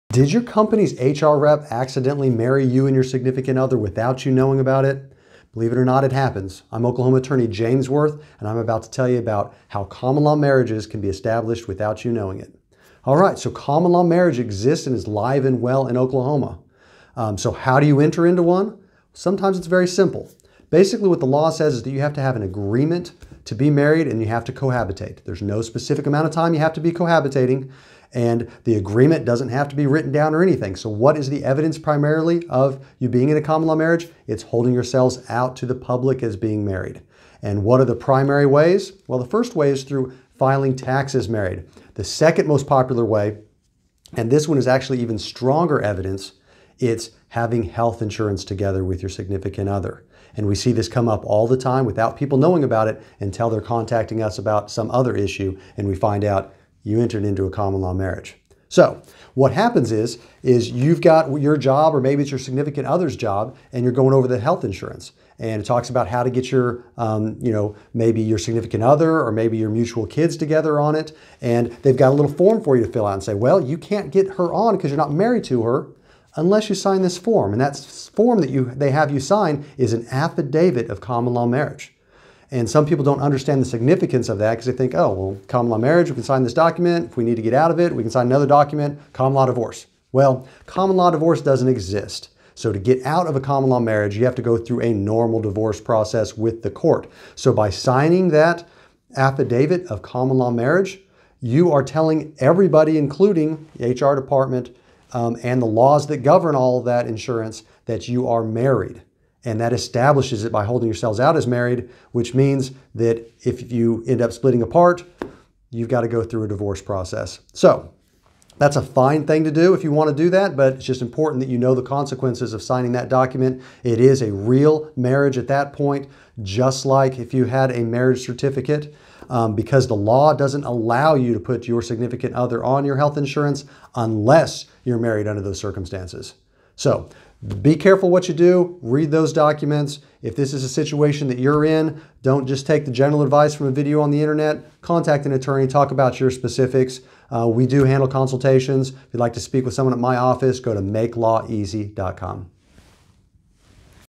Did HR Marry You and Your Significant Other?
Did your company's HR rep accidentally marry you and your significant other without you knowing about it? Believe it or not, it happens. I'm Oklahoma attorney James Wirth, and I'm about to tell you about how common law marriages can be established without you knowing it.